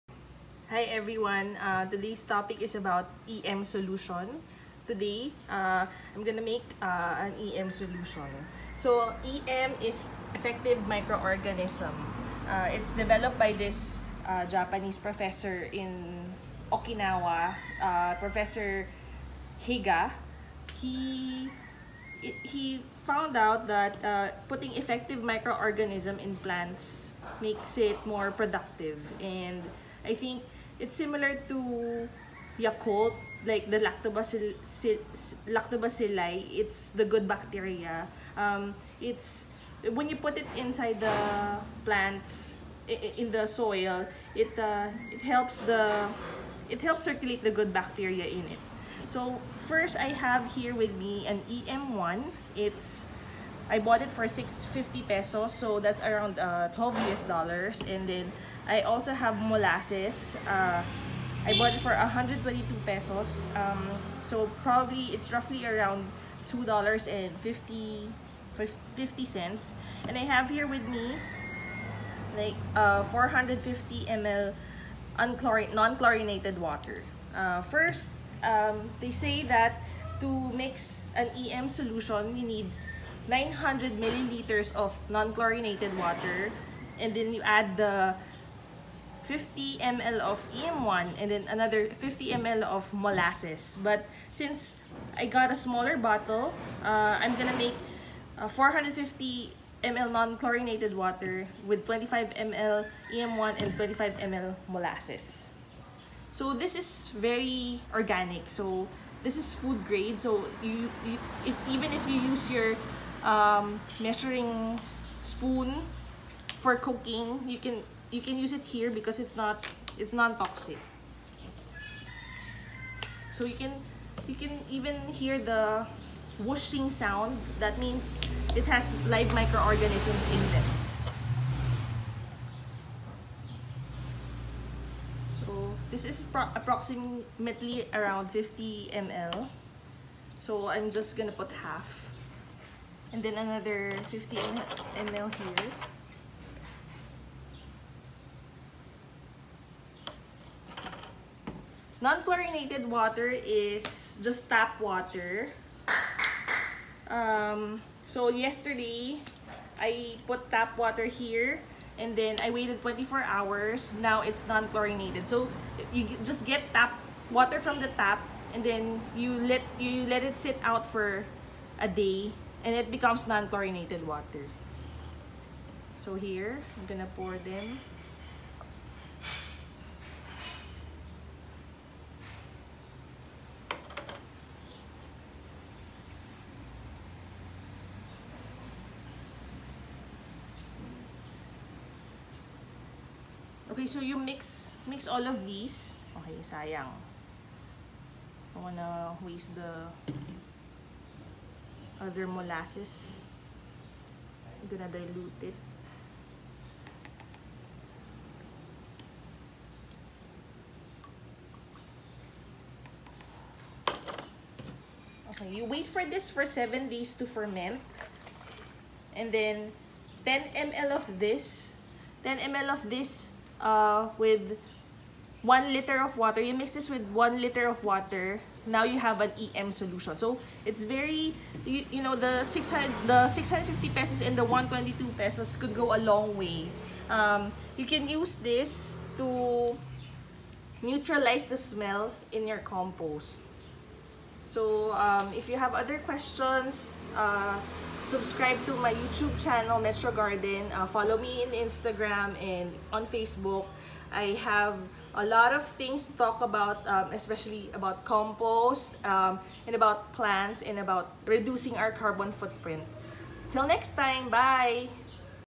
Secret Ingredient for Compost that does not Smell - EM Solution (Effective Microorganism)
EM Solution tutorial. This is good for neutralizing the smell of your compost. It can also help introduce nutrients to your plants.